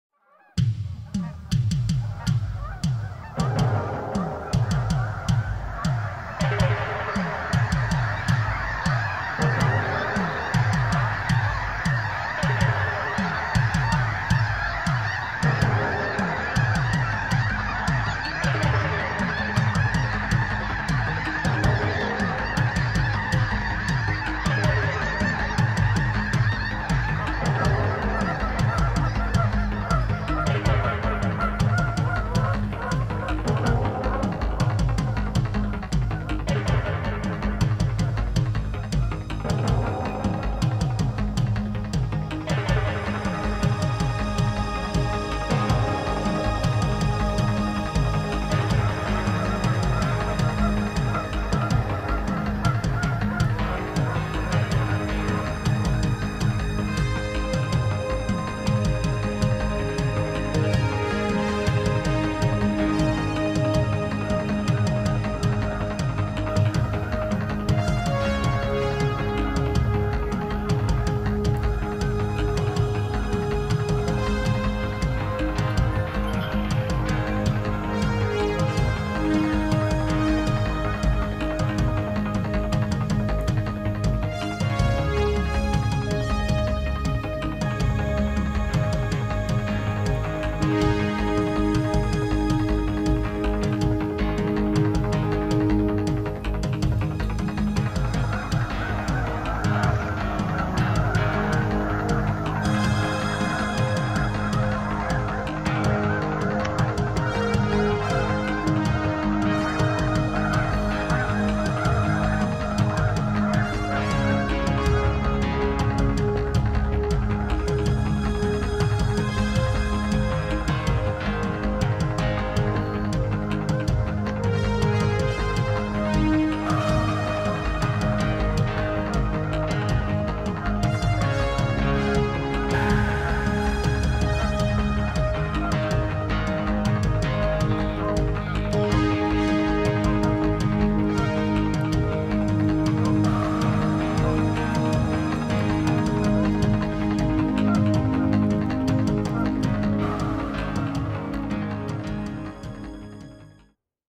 Migrating Geese in Sweden
Large swarms of migrating geese. Recorded at Lake Arriesjön near Malmö in Southern Sweden at Sep 27 2015. Destination unknown.